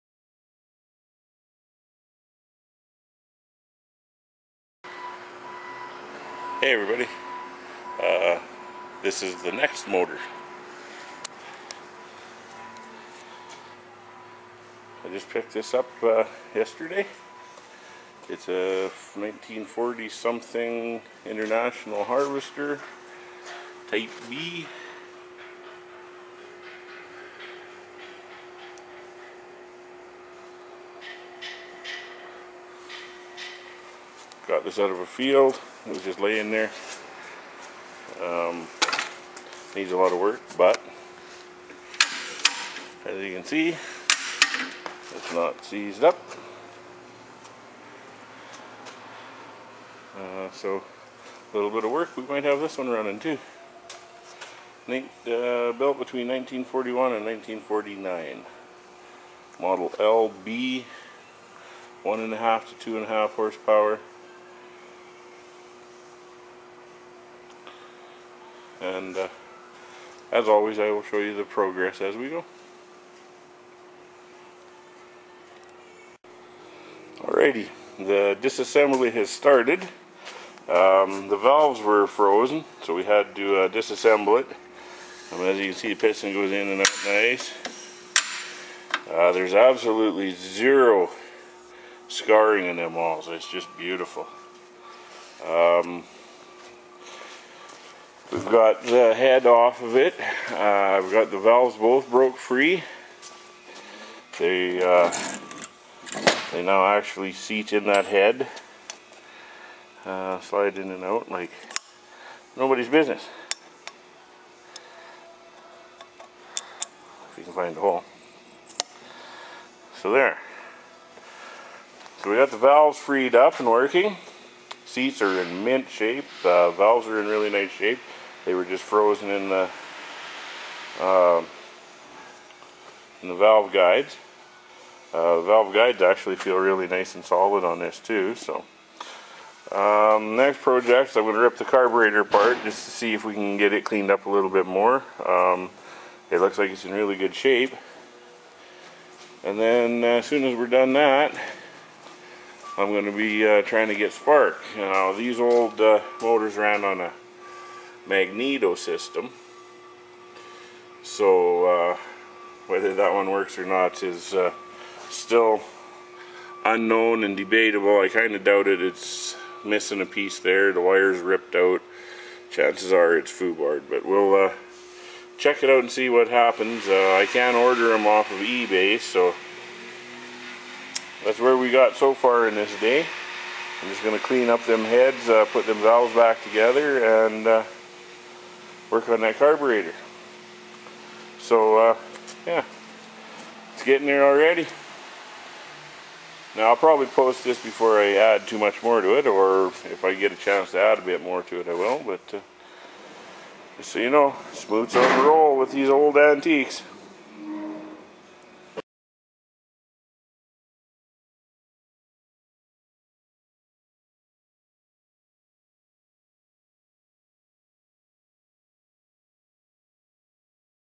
Type LB International Harvester Engine.
Smootts latest victim. A 40's something Type LB International Harvester Engine that I Picked up from the farm. Buried half way into the dirt. Forgotten. Now with a new chance at life.